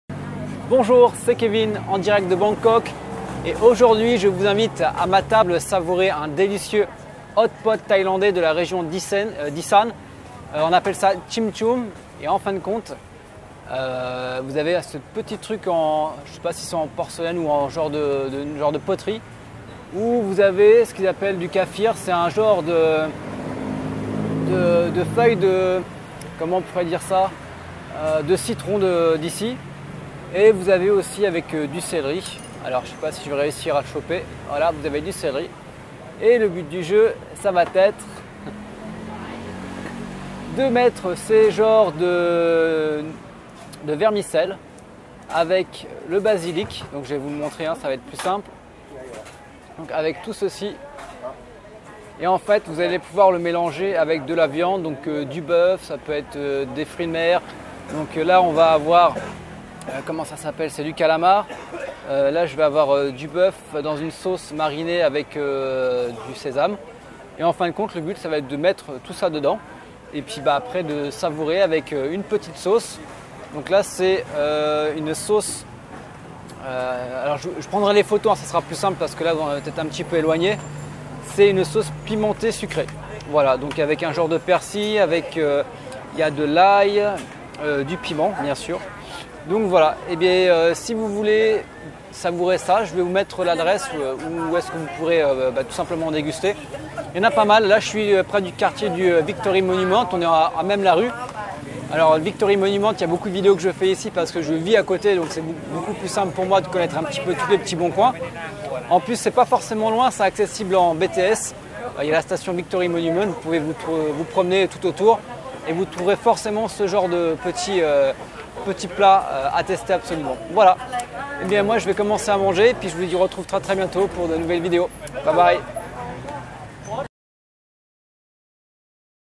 Jim jum la fondue thailandaise
Venez découvrir cette fondue venue directement de la région d'Issan. Il somptueux mélange de légumes, de viandes, de nouilles de riz, le tout accompagné d'une sauce sucrée-épicée.

Bref, pour moins de 5€ vous pourrez goûter à un plat authentique qui ravitaillera 2 personnes facilement.